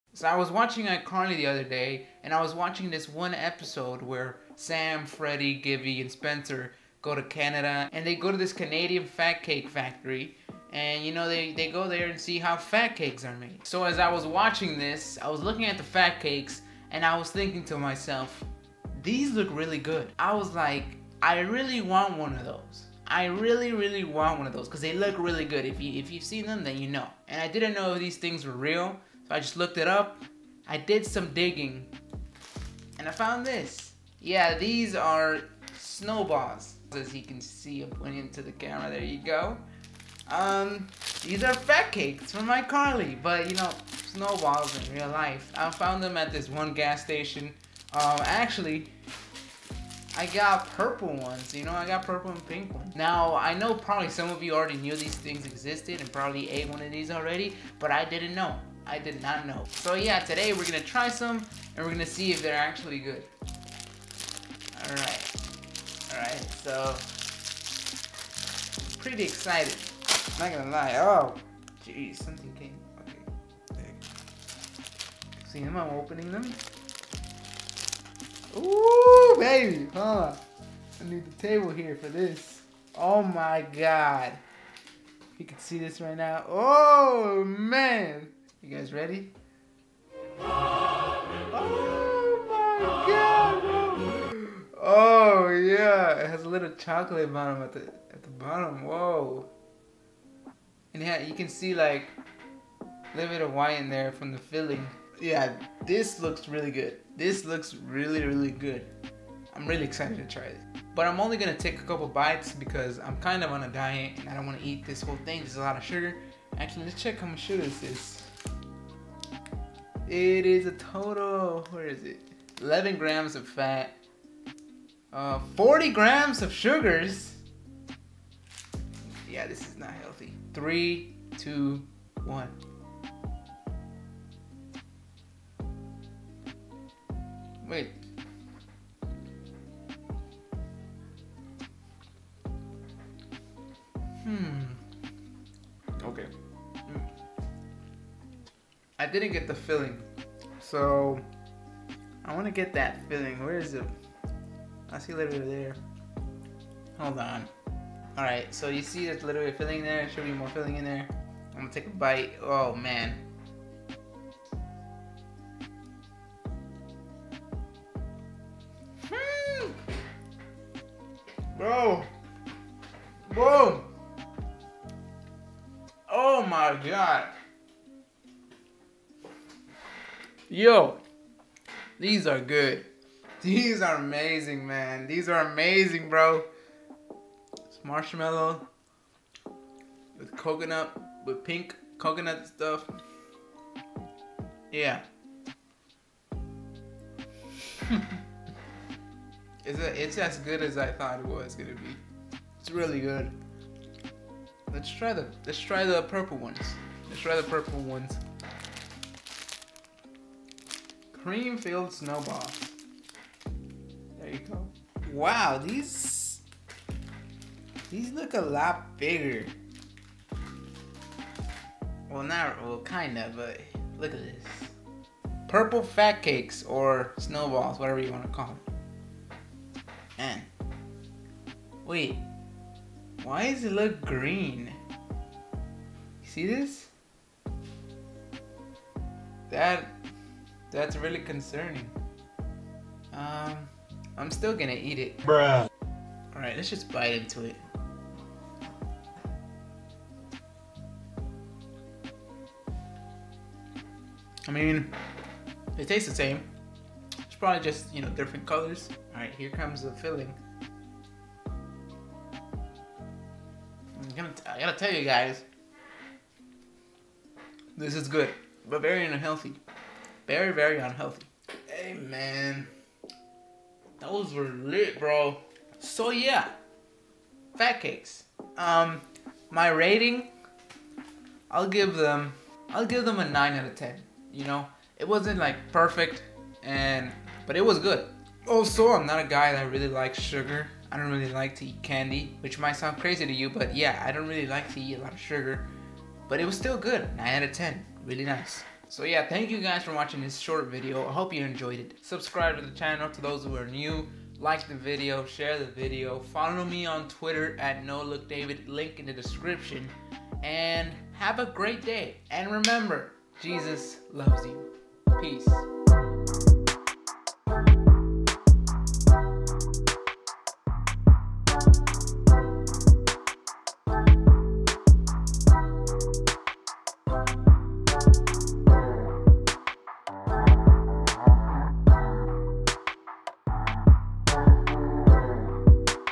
I found the fat cakes from iCarly..
Today, we eat some fat cakes.